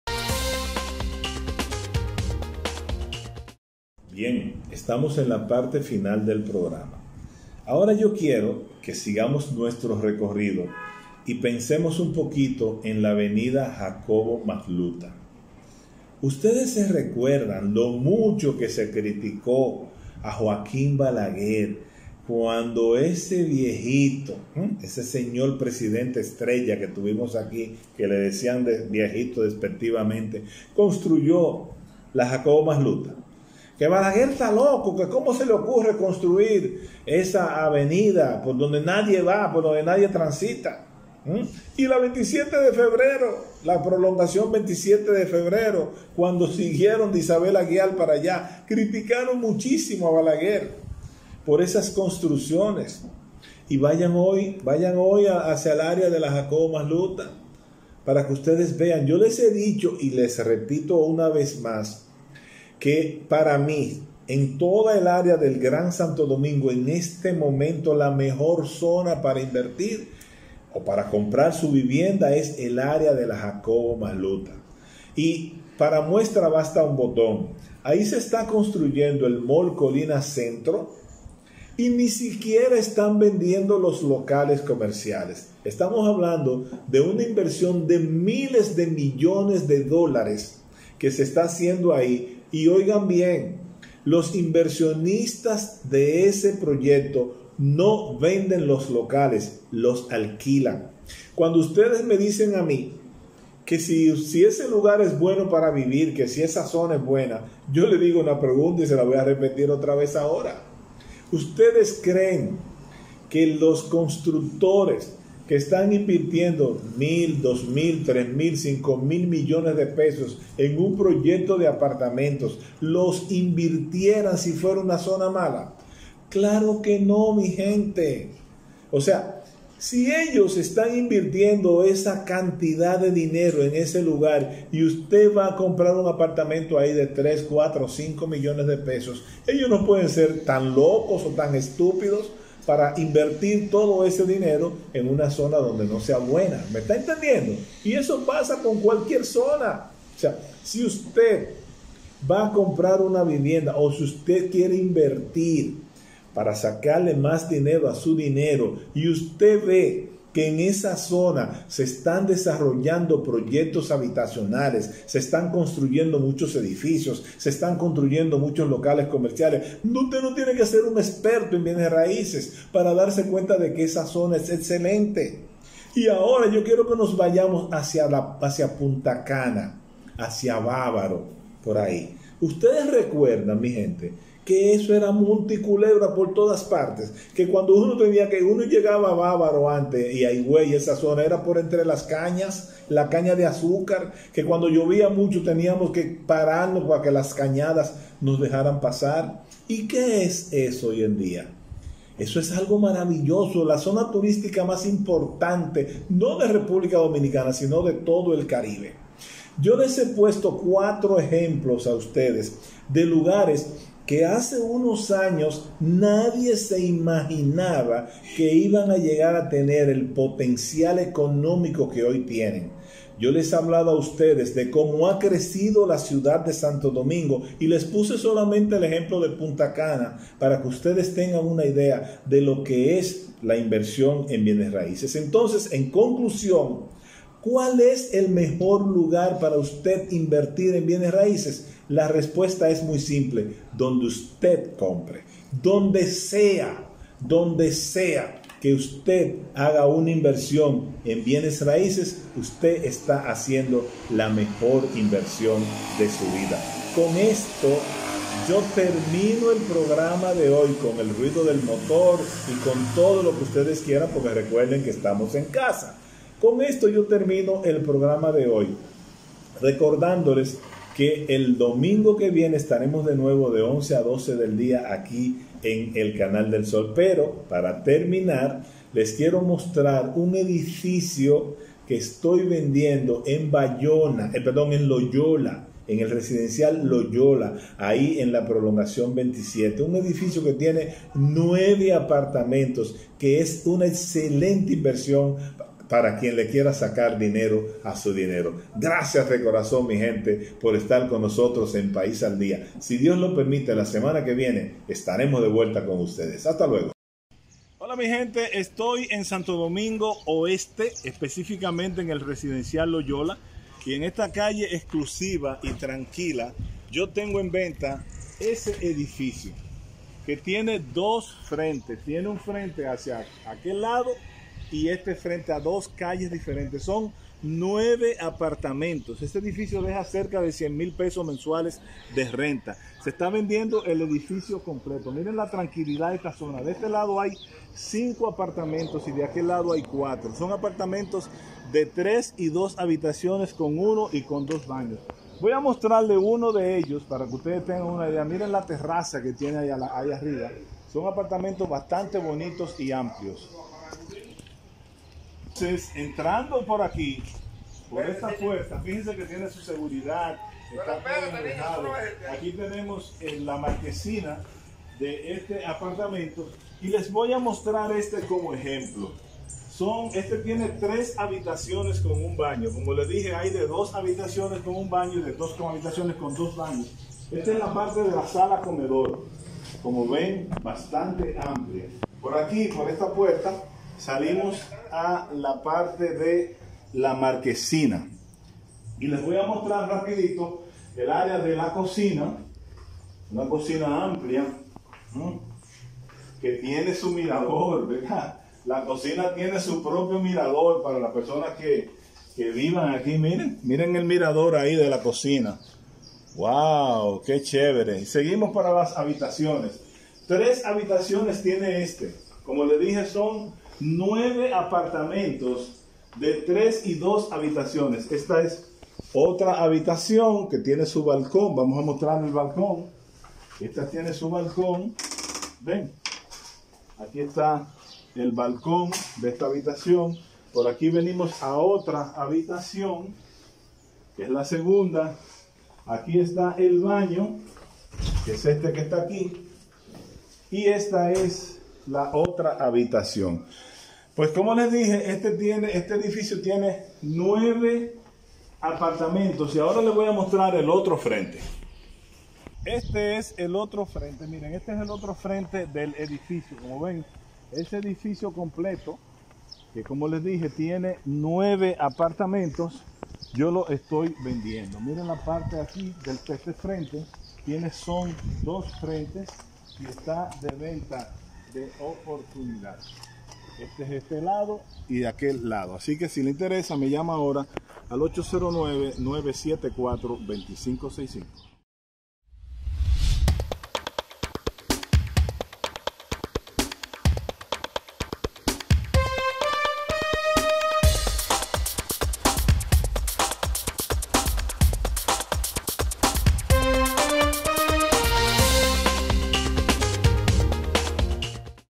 Edificio de 9 apartamentos. OPORTUNIDAD en Sto Dgo Oeste. Recuerdan lo que era la Jacobo Majluta?
Al momento de comprar o vender tu casa o apartamento, solar, etc,  lo más importante es recibir una buena Asesoría. Esa es mi especialidad.  Pregúntame.
Bendiciones.
César Fragoso